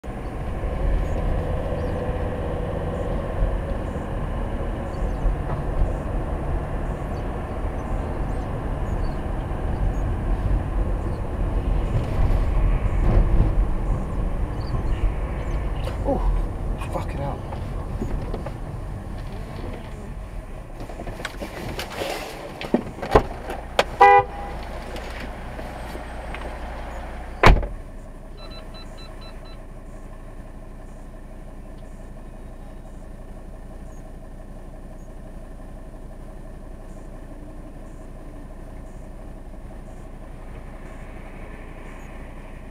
Dash cam footage of cyclist getting hit by car in Cambridge
The driver in front of me clearly didn't see the cyclist and immediately claimed that she did nothing wrong.

The cyclist didn't believe he did anything wrong but was still in shock so couldn't quite recall.  I was in the car behind, but I only just caught a glimpse of the poor chap getting hit, so I couldn't say for sure.

Luckily, the dash cam caught the whole thing.  (Please excuse the potty mouthed reaction...!)

I couldn't tell if the driver was about to drive away so I honked to get her attention, before seeing to the cyclist.

Nextbase 512g Ultra | Dash Cam video footage.  Accident on Newmarket Road, Cambridge.  Monday 25th April 21:29